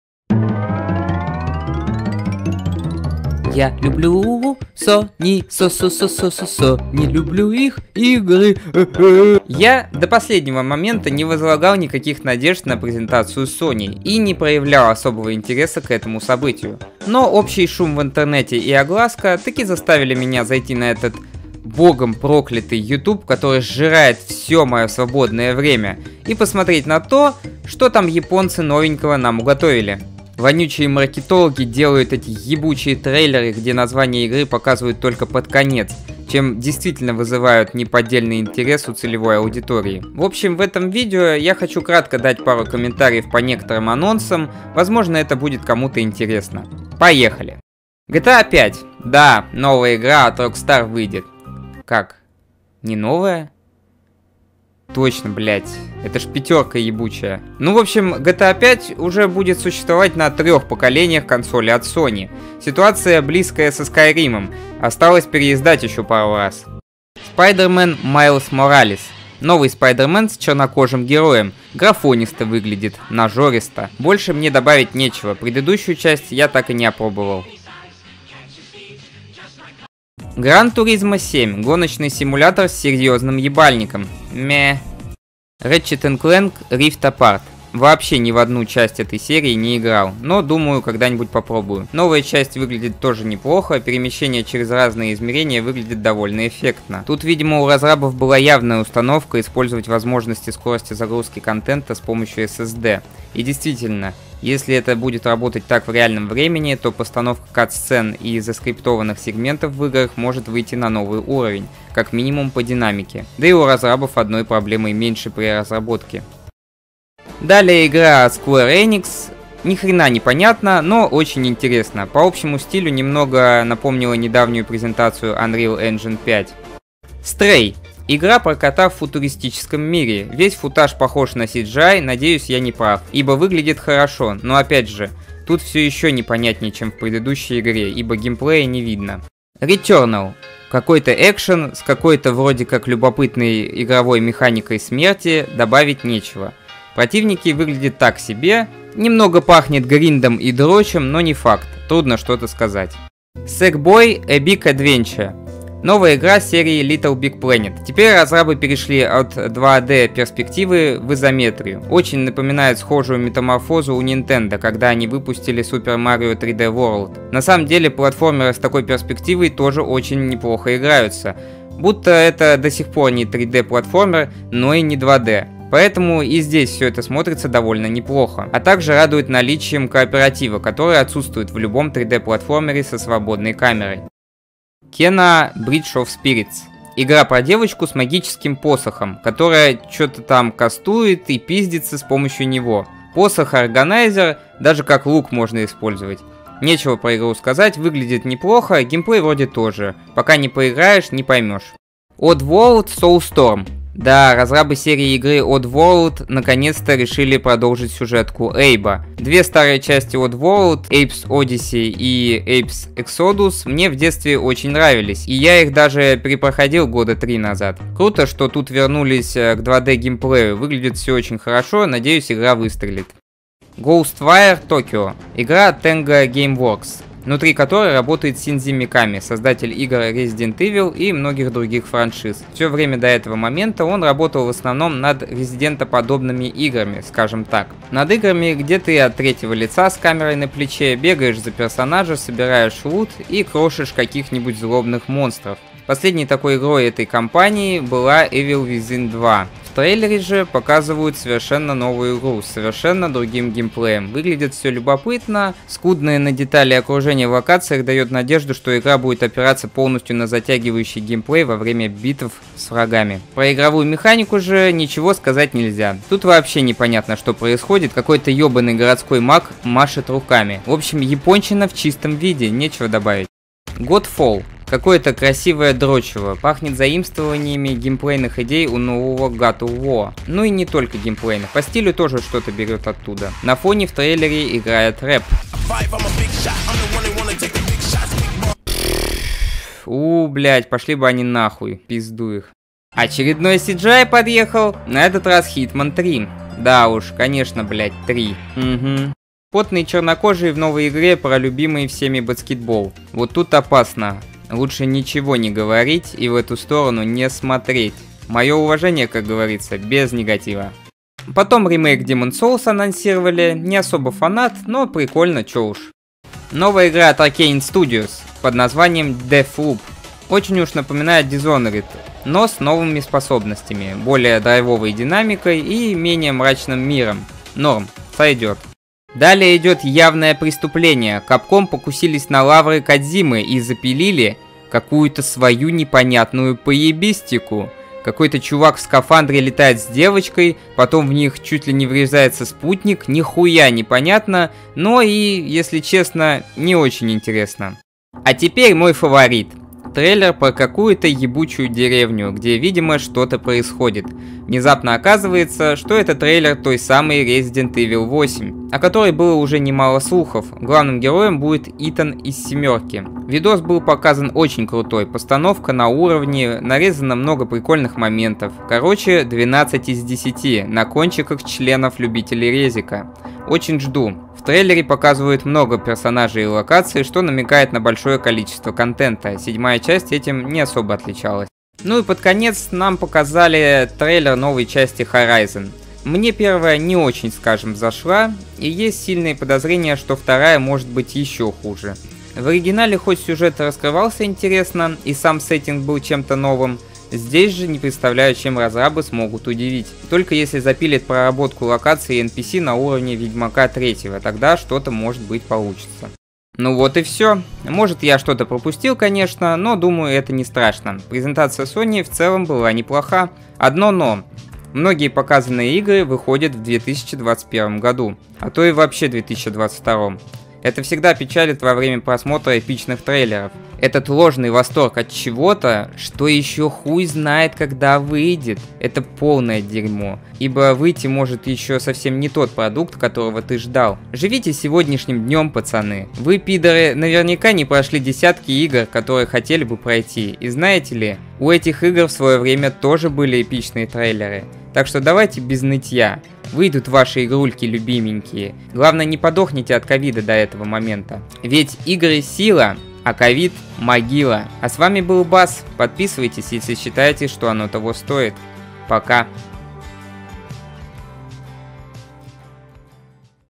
Презентация PS5 прошла НОРМАЛЬНО
Мои комментарии по поводу вчерашней презентации.
Поддержите лайками и комментариями, если интересен подобный контент.